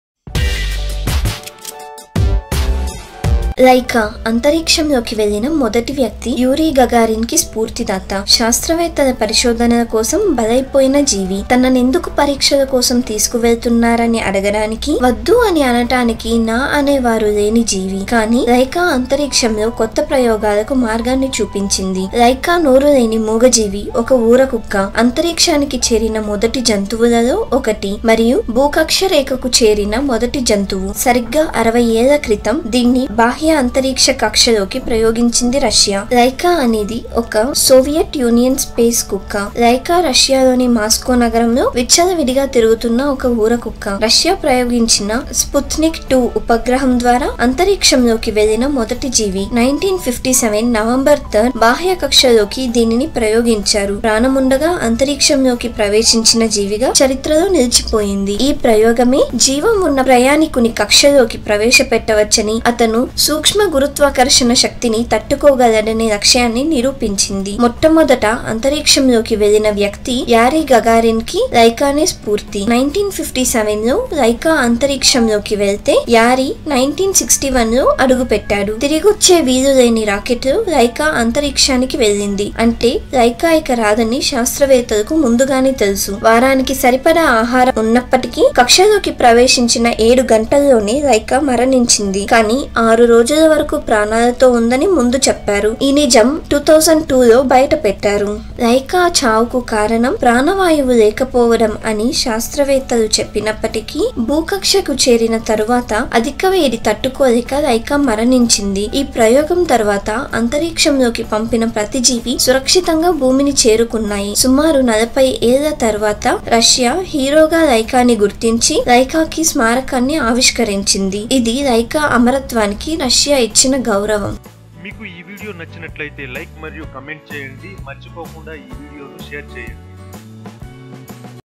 అంతరీక్ష కుక్క లైకా గురించి ఆసక్తికర సమాచారం మీకోసం | Interesting Facts About First Space Dog Lyka
Layka, Laika is the first space dog sent to space by russia.
was a Soviet space dog who became one of the first animals in space, and the first animal to orbit the Earth. Laika, a stray dog from the streets of Moscow, was selected to be the occupant of the Soviet spacecraft Sputnik 2 that was launched into outer space on November 3, 1957.